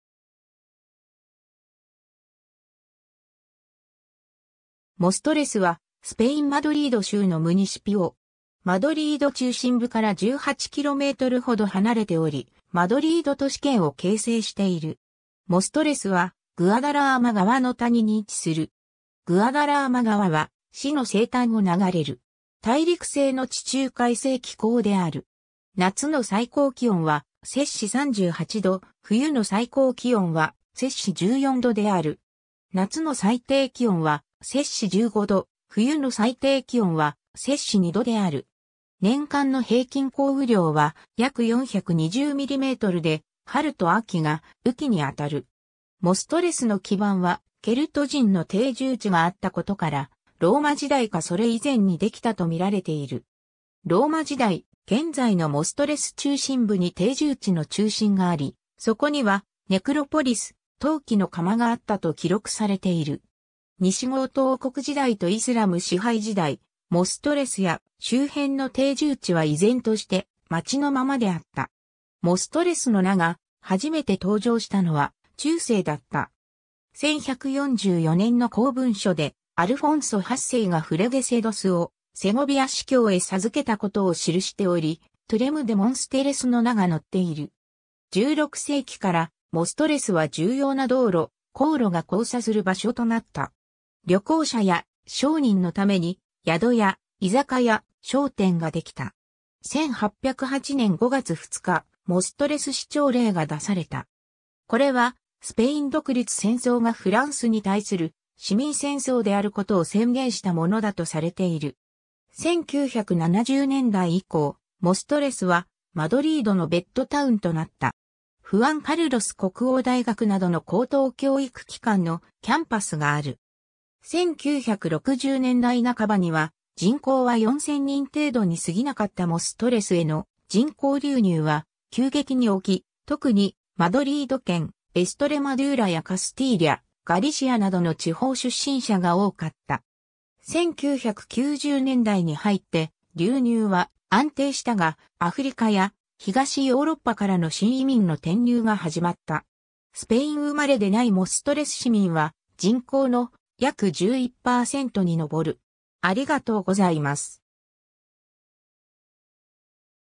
モストレス
モストレス（Móstoles）は、スペイン・マドリード州のムニシピオ（基礎自治体）。
マドリード中心部から18kmほど離れており、マドリード都市圏を形成している。
 モストレスはグアダラーマ川の谷に位置する。
グアダラーマ川は市の西端を流れる。
 大陸性の地中海性気候である。
夏の最高気温は38℃、冬の最高気温は14℃である。
夏の最低気温は15℃、冬の最低気温は2℃である。
年間の平均降雨量は約420mmで、春と秋が雨季にあたる。
 モストレスの基盤は、ケルト人の定住地があったことから、ローマ時代かそれ以前にできたと見られている。
ローマ時代、現在のモストレス中心部に定住地の中心があり、そこにはネクロポリス、陶器の窯があったと記録されている。
 西ゴート王国時代とイスラム支配時代、モストレスや周辺の定住地は依然として町のままであった。
 モストレスの名が初めて登場したのは中世だった。
1144年の公文書で、アルフォンソ8世がフレゲセドス（現在のロランカとフエンラブラーダにまたがっていた村）をセゴビア司教へ授けたことを記しており、トゥレム・デ・モンステレス（Turrem de Monsteles）の名が載っている。
 16世紀から、モストレスは重要な道路・行路が交差する場所となった。
旅行者や商人のために、宿屋、居酒屋、商店ができた。
 1808年5月2日、モストレス市長令（es）が出された。
これは、スペイン独立戦争がフランスに対する市民戦争であることを宣言したものだとされている。
 1970年代以降、モストレスはマドリードのベッドタウンとなった。
フアン・カルロス国王大学などの高等教育機関のキャンパスがある。
 1960年代半ばには人口は4,000人程度にすぎなかったモストレスへの人口流入は、急激に起き、特にマドリード県、エストレマドゥーラやカスティーリャ、ガリシアなどの地方出身者が多かった。
1990年代に入って流入は安定したが、アフリカや東ヨーロッパからの新移民の転入が始まった。
スペイン生まれでないモストレス市民は、人口の約11％にのぼる。